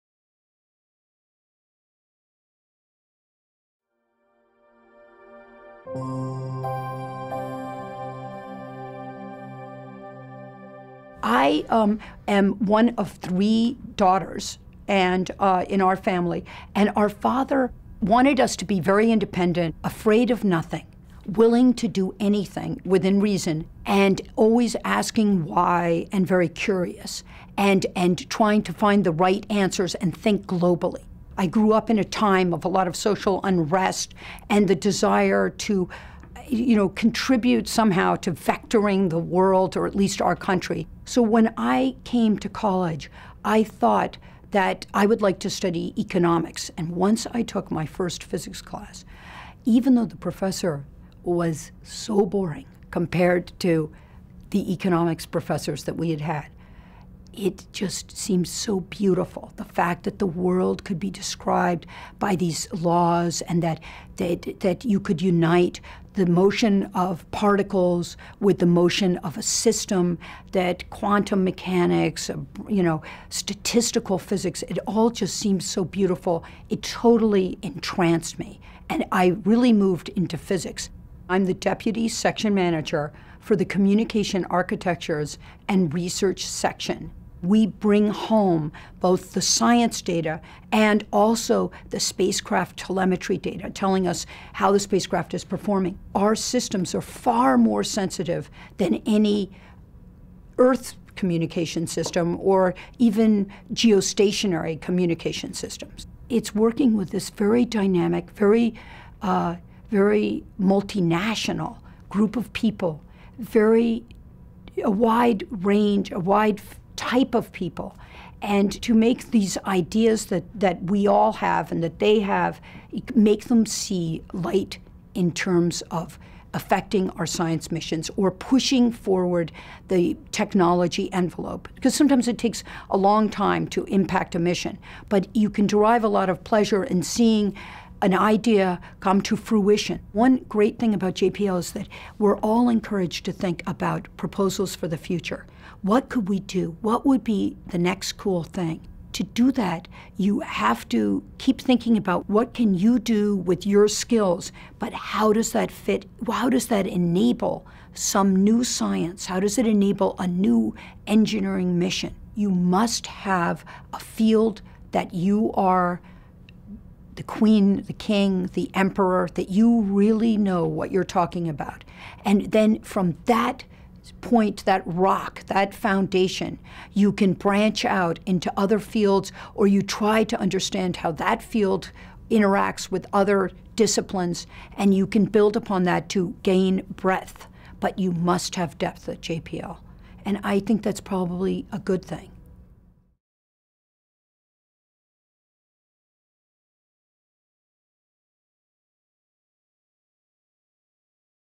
Women at JPL - Polly Estabrook, Telecommunications Engineer
These are the stories of the remarkable women at NASA's Jet Propulsion Laboratory who are pushing the boundaries of human knowledge as they explore the unknown. Leaders in science, technology, engineering and mathematics, these inspirational women have achieved great successes and faced challenging odds to become the role models they are today.